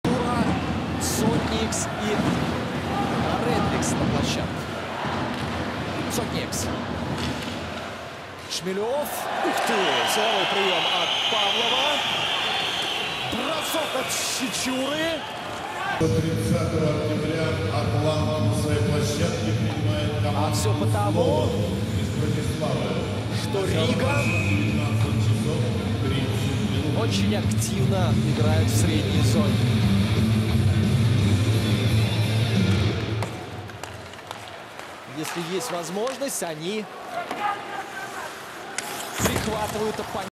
Павлов ловит Шмелева на силовой / Pavlovs hammers Shmelyov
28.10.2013, Виталий Павлов здорово отловил соперника в нейтральной зоне